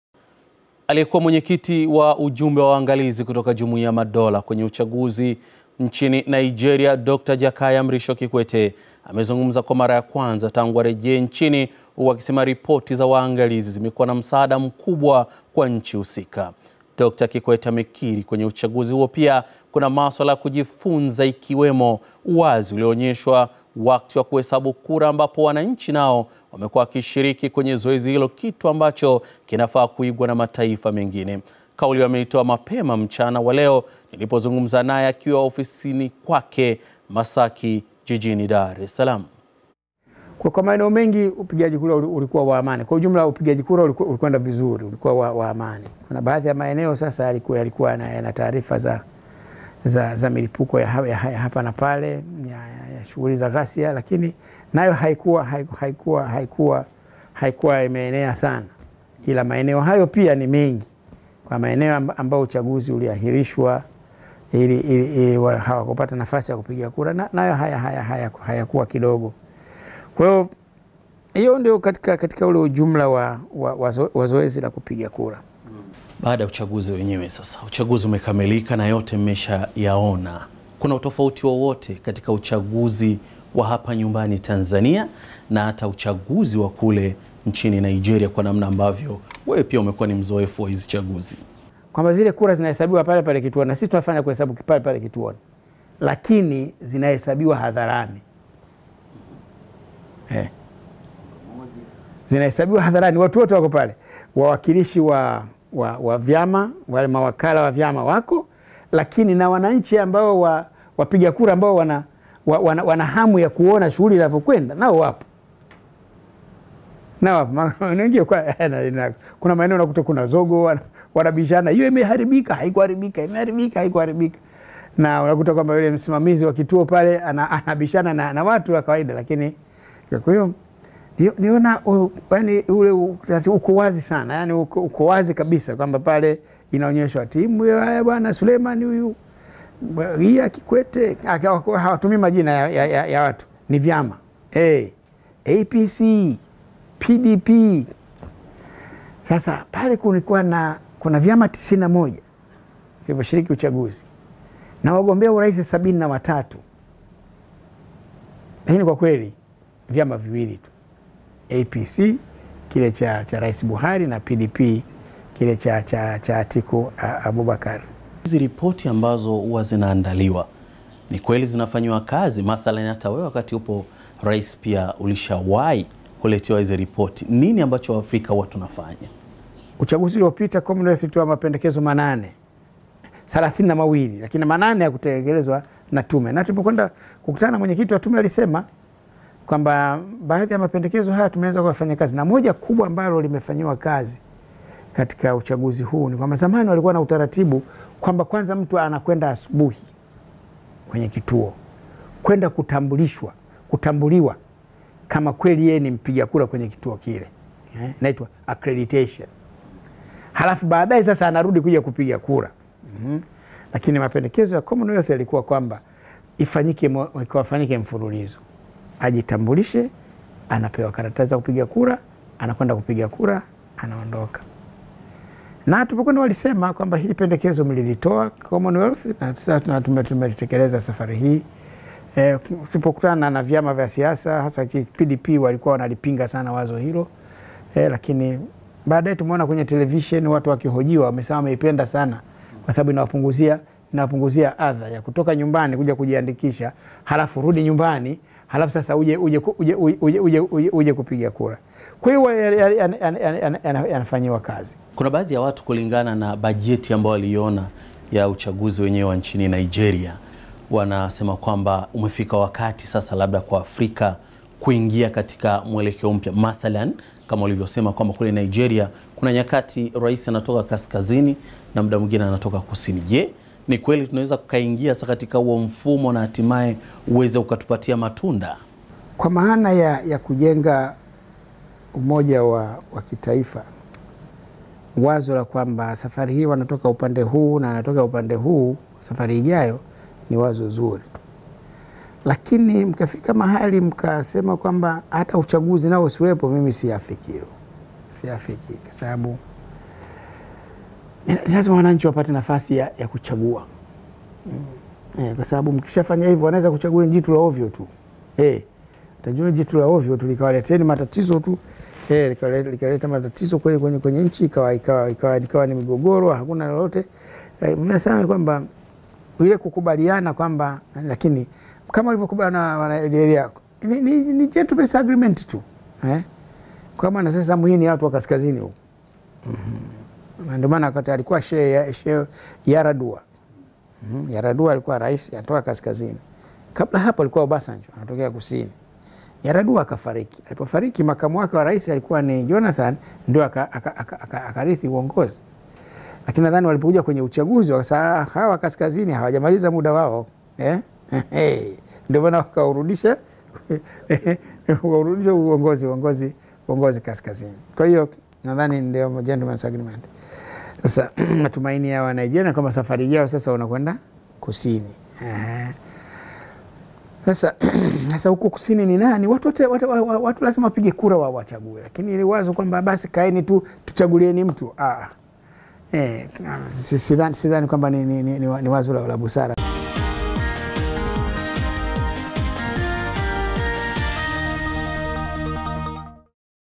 DKT JAKAYA KIKWETE AELEZEA UCHAGUZI WA NIGERIA
Mahojiano ya kwanza niliyofanya na Rais Mstaafu wa Tanzania ambaye alikuwa ni Mwenyekiti wa Waangalizi wa Jumuiya ya Madola kwenye Uchaguzi wa Nigeria Dokta John Magufuli akieleza namna mambo yalivyoenda pamoja na kile ambacho amejifunza!!